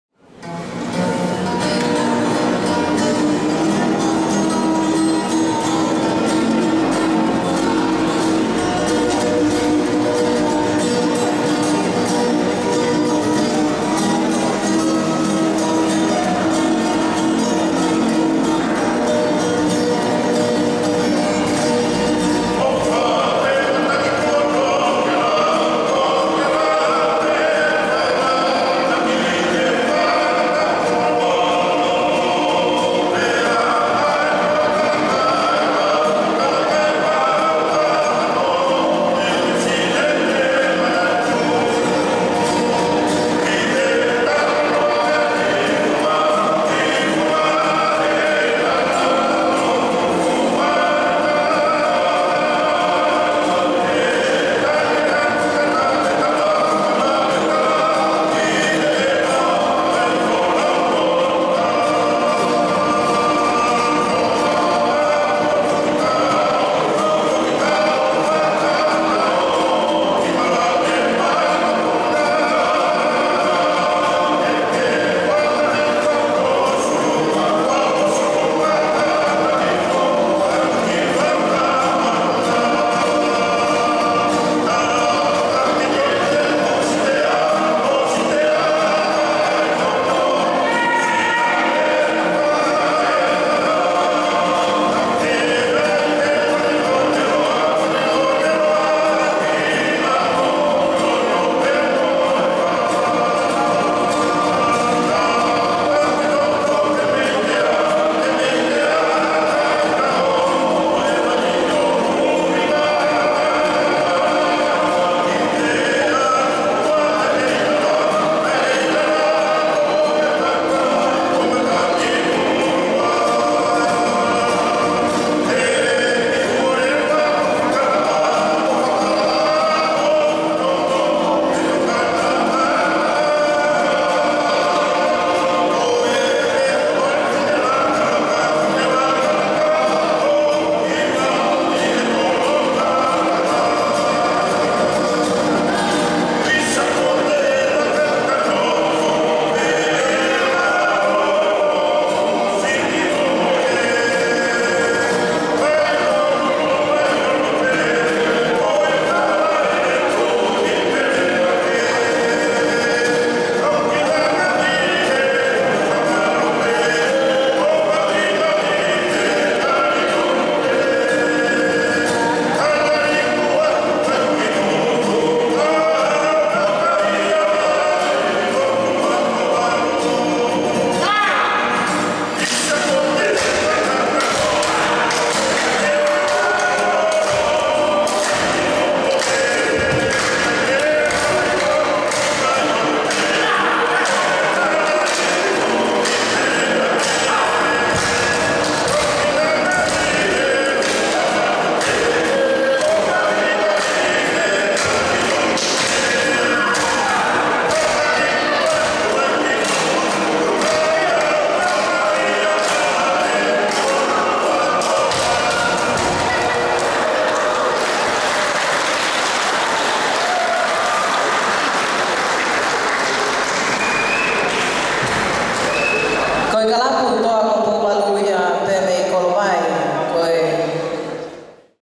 Uike Heilala 2014 - Kalapu Toa Ko Pouvalu 2
Fe'auhi Sivi Hiva Kalapu Kava Tonga Heilala 2014 @ Queen Salote Memorial Hall Kingdom Of Tonga 8th July 2014